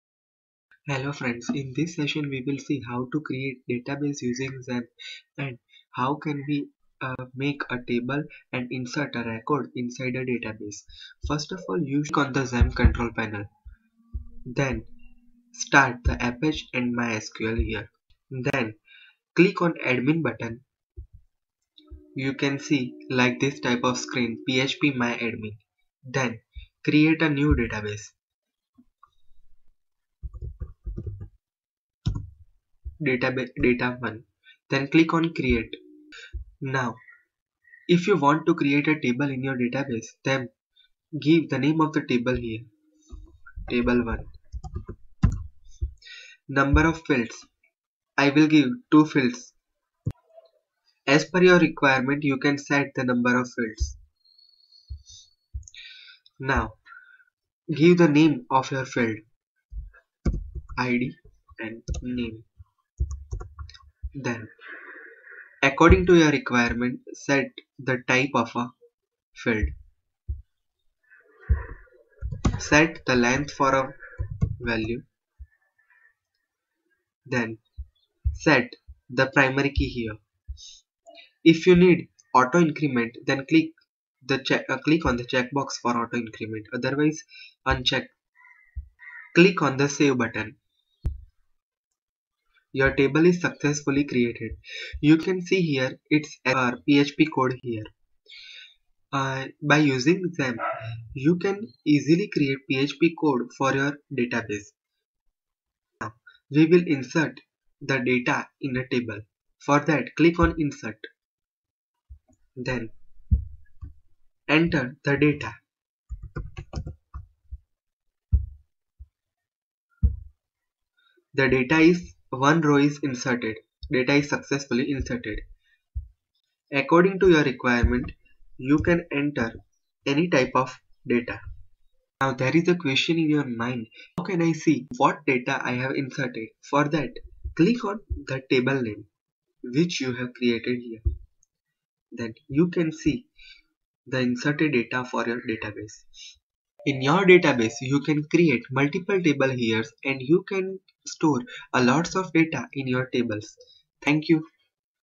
How to create database in XAMPP using phpMyAdmin ?
In this Video I have explained ... How to create database for your website in XAMPP  using phpMyAdmin .
I hope everyone like it!!